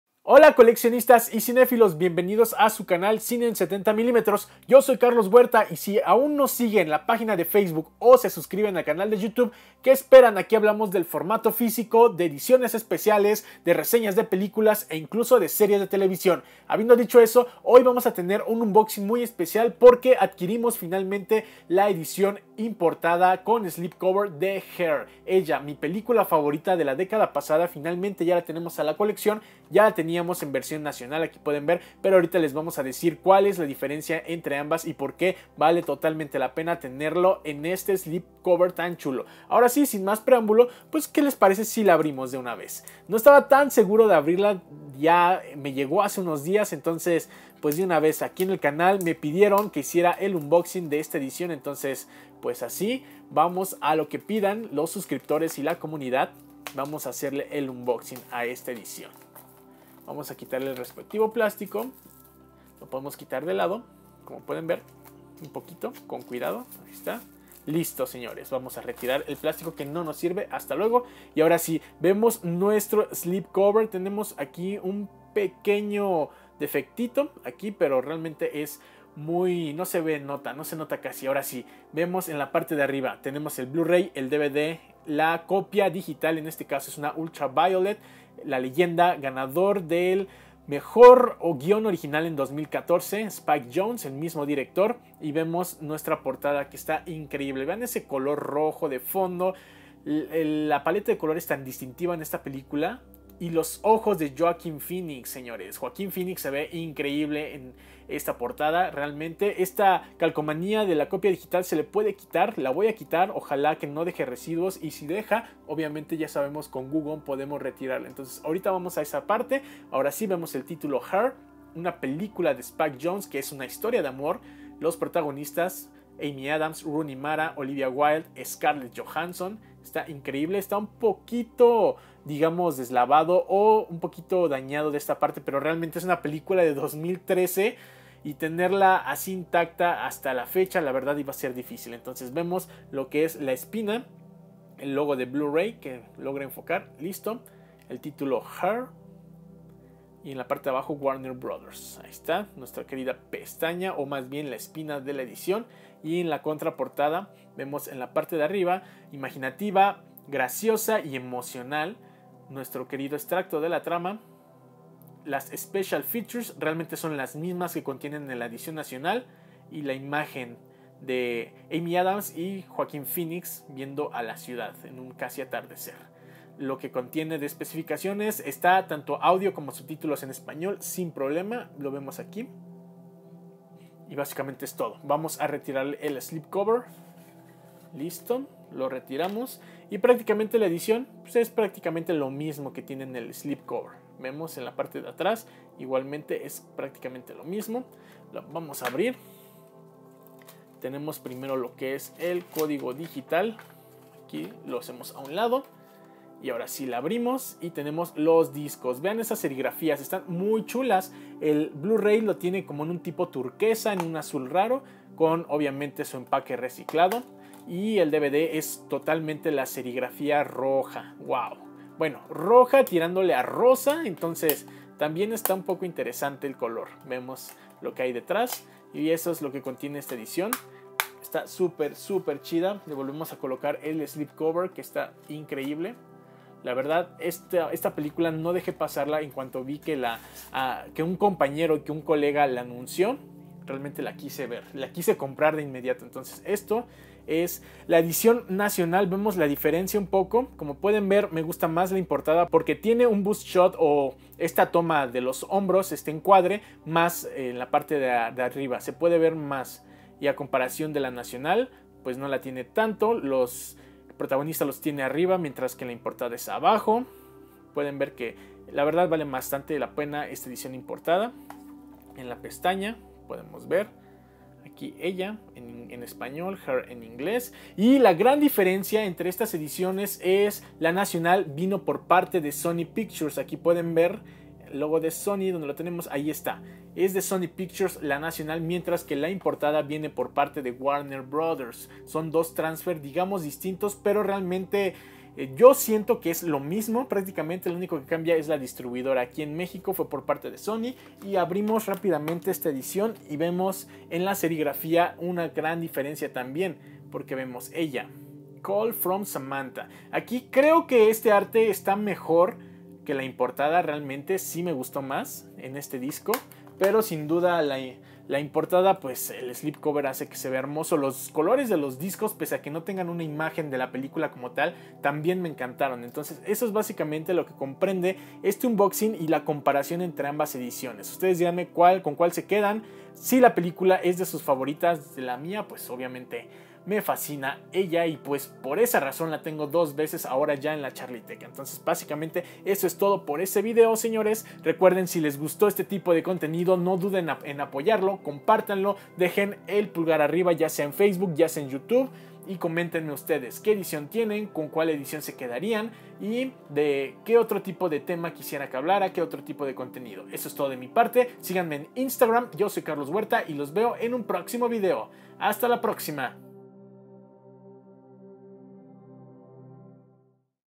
Her (ELLA)| Unboxing Blu-Ray Slipcover por Cine en 70MM
En este martes de Unboxing les comparto mi cinta favorita de la década pasada en su versión importada, ya en la charlieteca.

No olviden de dejar su like y comentar, qué más quisieran ver en el canal comunidad.